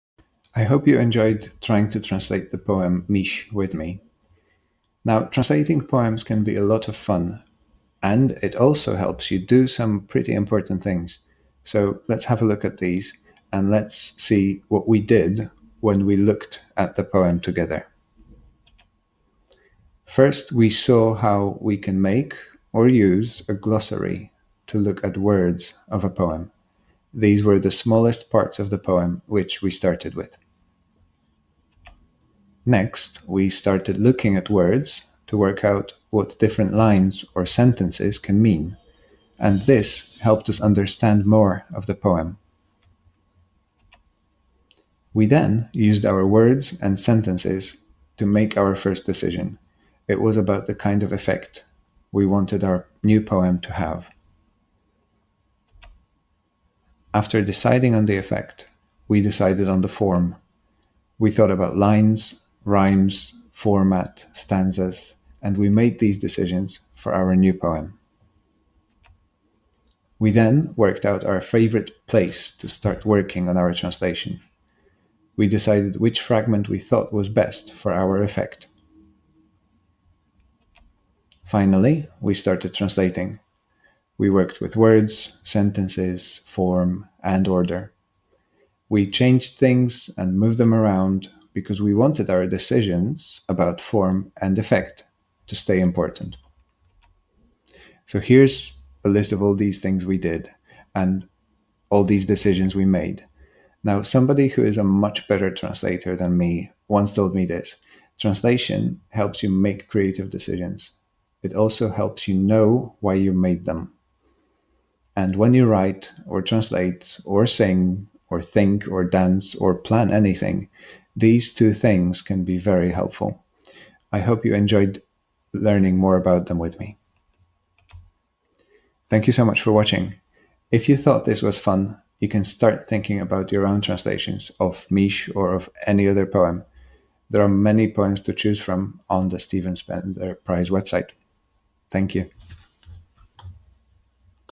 Polish Spotlight - How to translate a Polish poem 5
Part 5 of a short series of videos aimed at budding young translators. Translate a poem into English and enter the Stephen Spender Prize!